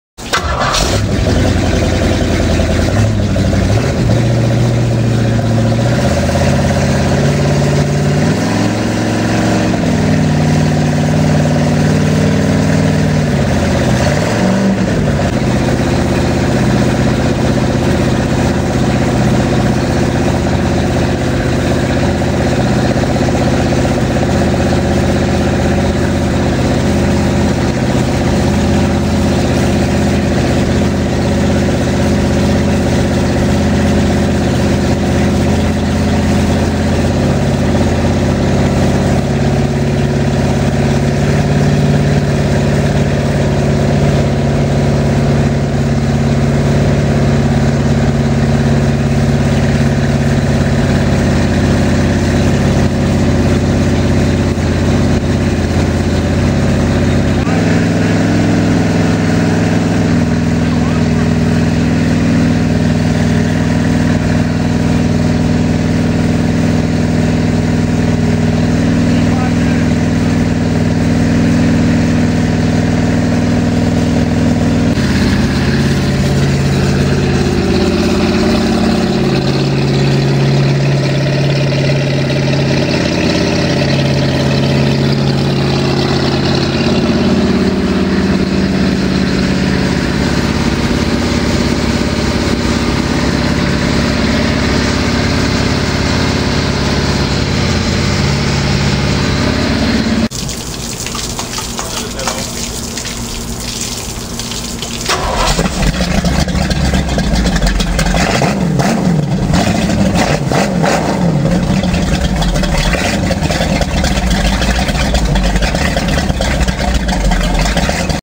Oldmobile 455 Tahiti jet boat olds camshaft break in
First fire of the marine Oldsmobile 455. Things went fantastic. The entire build of this engine can be seen on my channel.